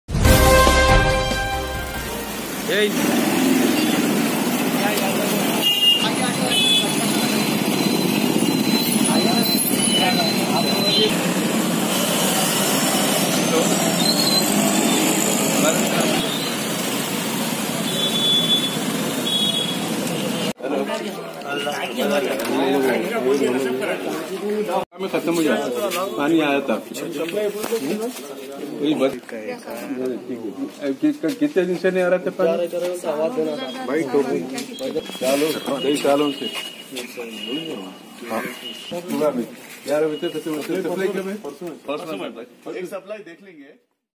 Syed Ahmed pasha Qadari visited to Moghalpura area with Water department and Ghmc officers.
Aimim MLA charminar  Syed Ahmed pasha Qadari  visited to Moghalpura area with Water department and Ghmc officials on this occasion Moghalpura corporator Abbu Rahil also Present with mujju Patel Aimim active member